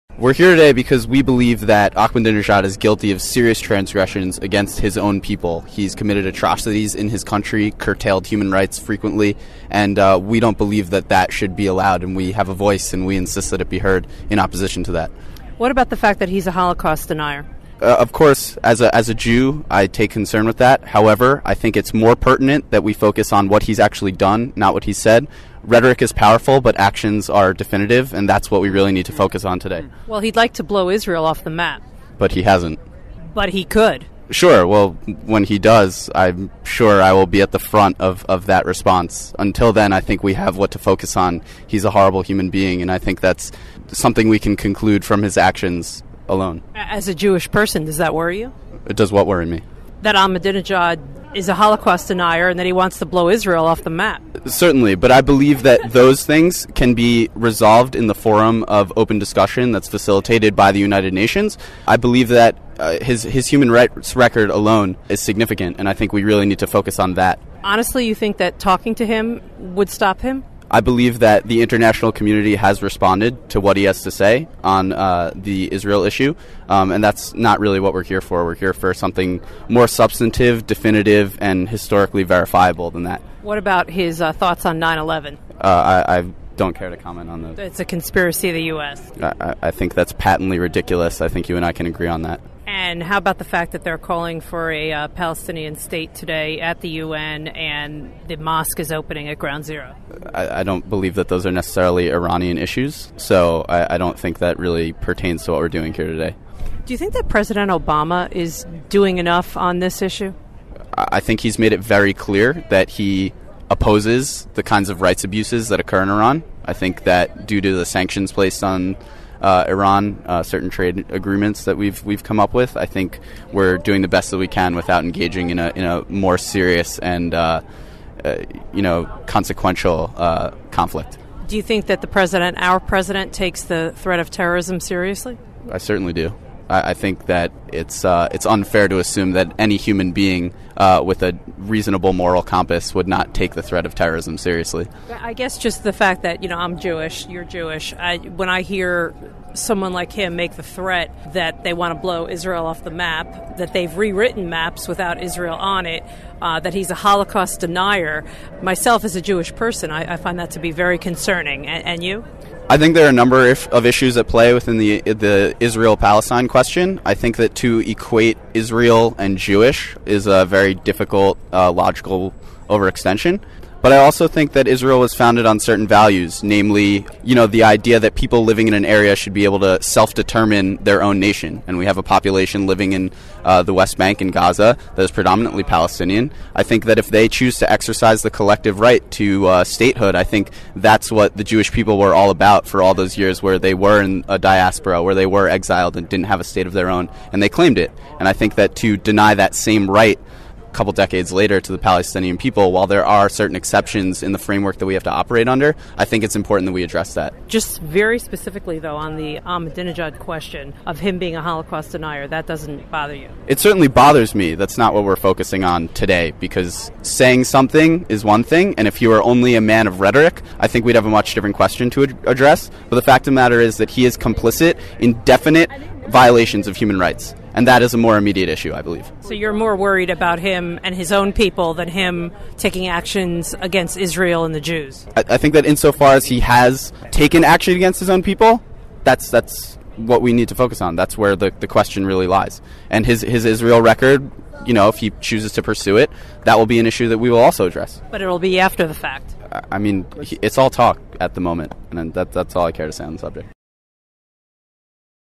Columbia Students Protesting Iran On Human Rights
Robyn Walensky from TheBlaze interviews Jewish students at Columbia University